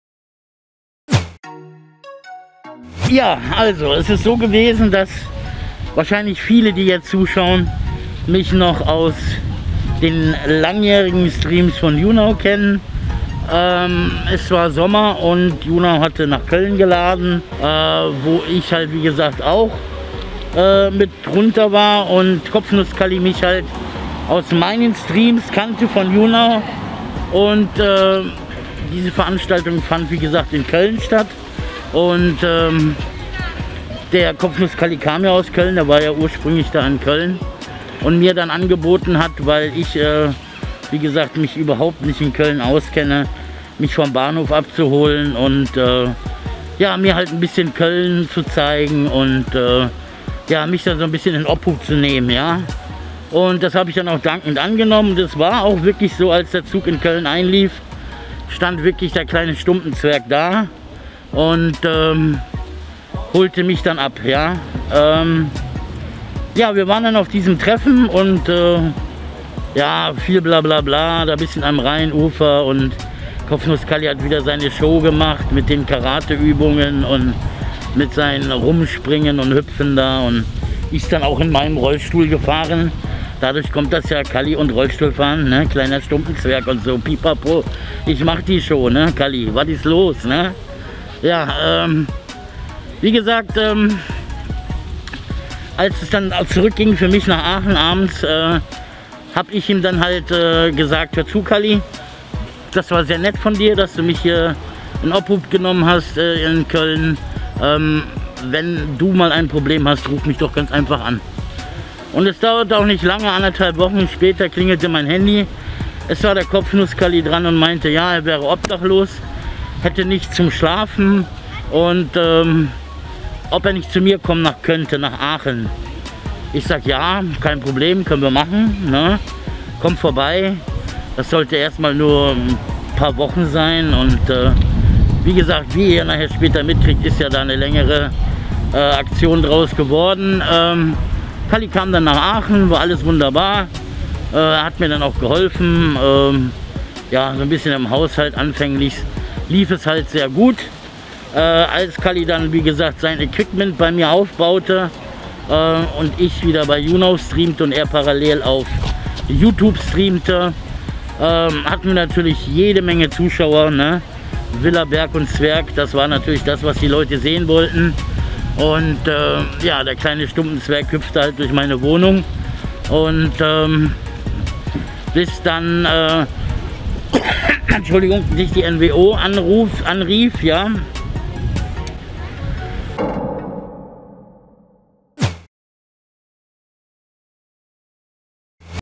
Der ERSTE Kontakt zu KopfnussKalli 🌟 - BobTV #2
BobTV hat eine gemeinsame Vergangenheit mit KopfnussKalli. Diese ist nun vorbei. Heute bekommen wir einen Einblick in die Anfänge der ganzen Geschichte.

0:00 Der ERSTE Kontakt
01:05 Als Duo Unterwegs
01:34 Das Verhängnisvolle Angebot
02:31 Der Anfang vom Ende

Musik prod. by Skagbeats